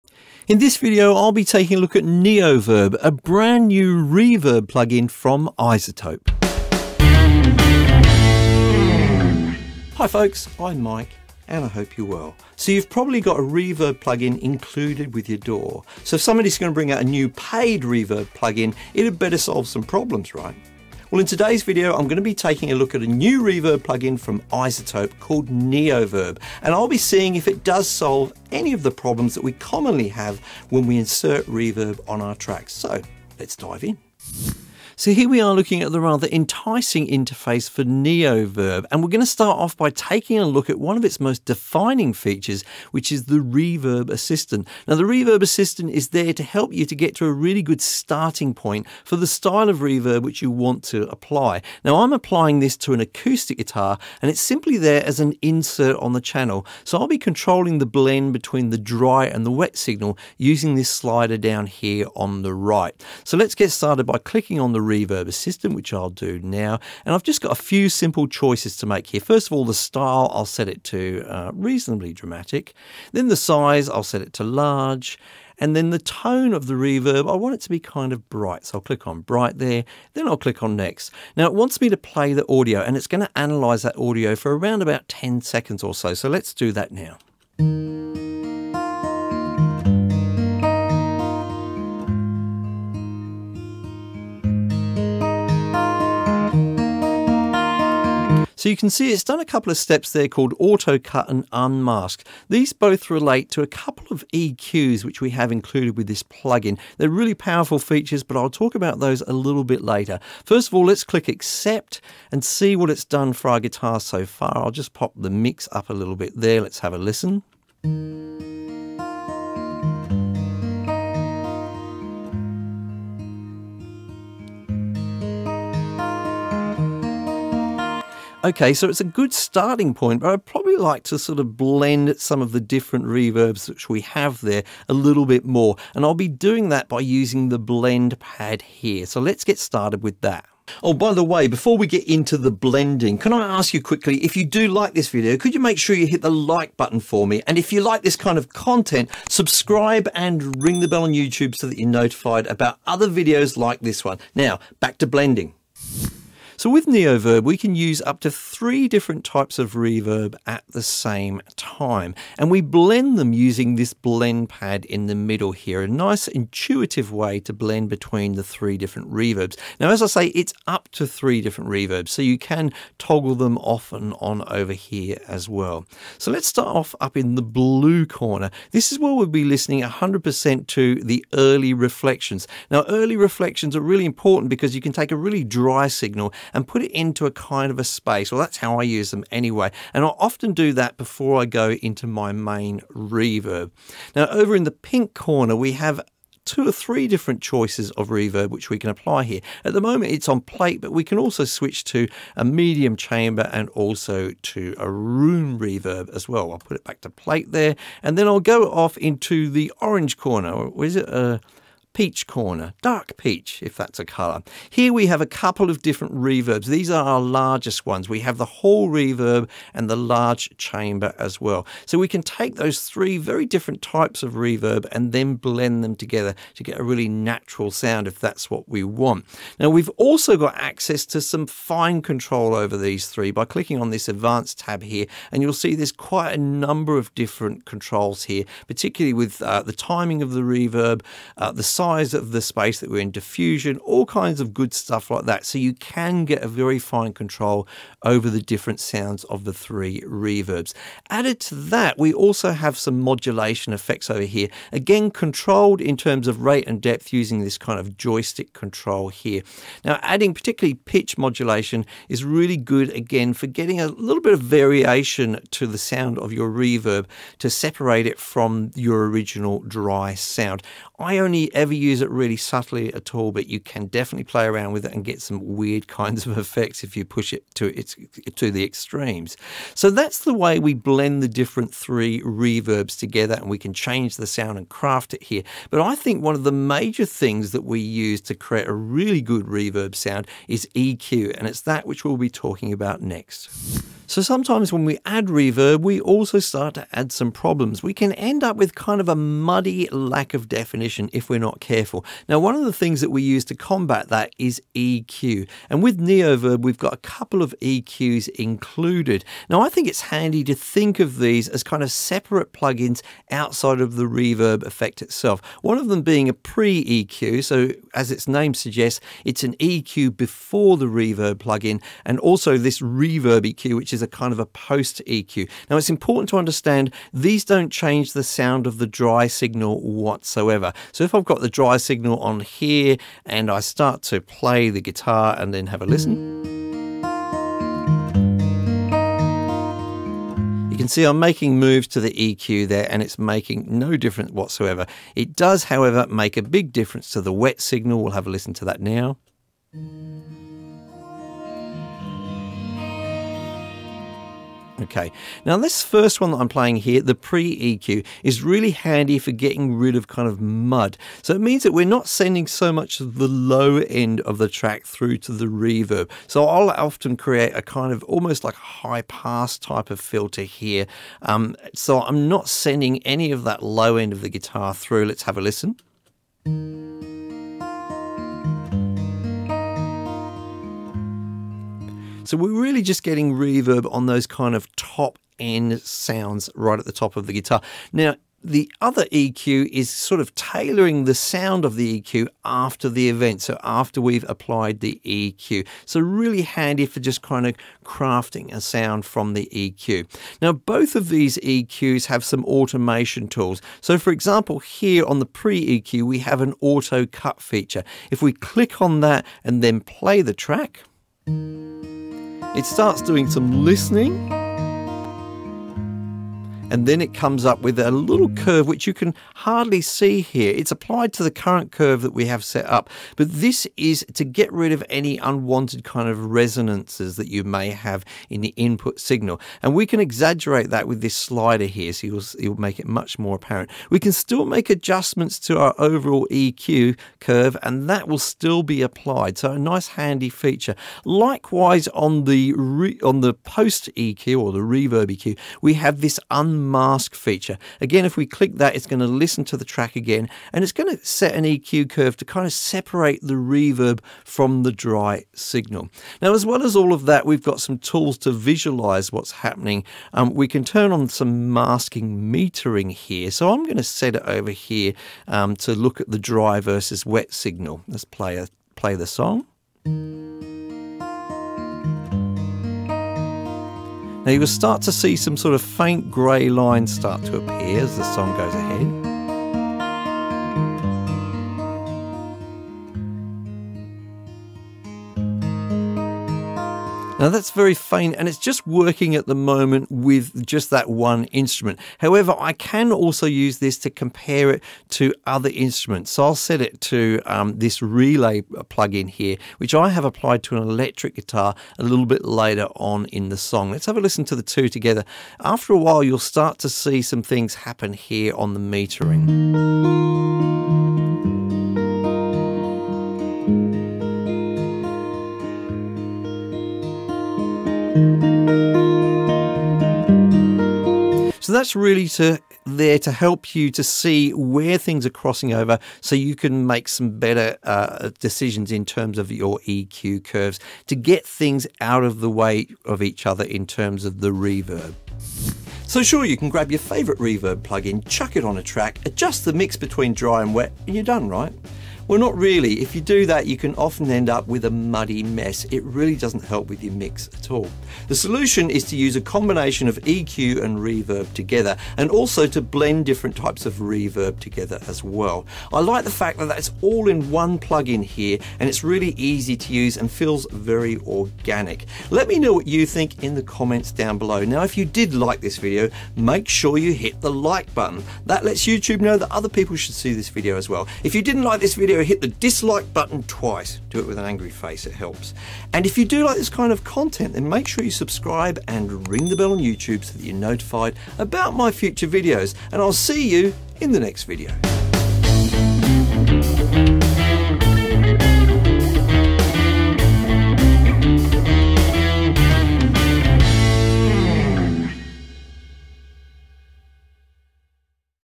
iZotope Neoverb - Why do you need it? - First look and Review
In this video, I'll be taking a look at Neoverb, a brand new reverb plugin from iZotope!